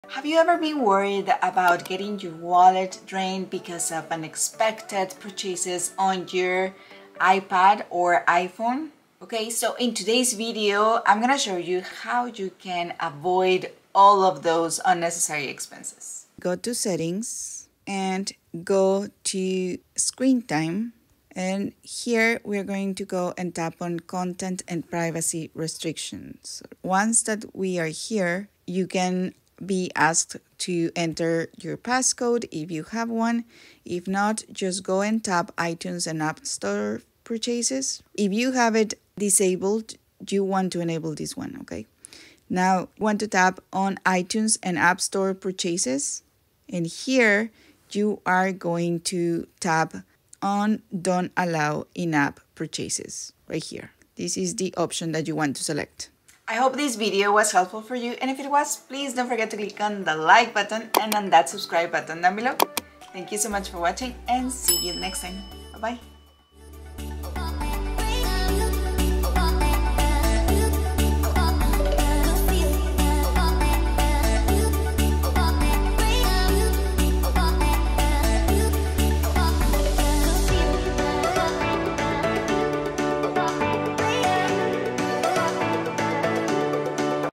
How to turn off in app purchases on iPad | Ipad Parental Controls
today I will show you How to turn off in app purchases on iPad, this will have with Parental controls and avoid get unnecessary expenses or unexpected charges on your credit card.

✹ Equipment used on video:
MEMORY CARD
Iphone Adapter Headphone

✹ EDITING SOFTWARE:
FINAL CUT PRO
EDITING COMPUTER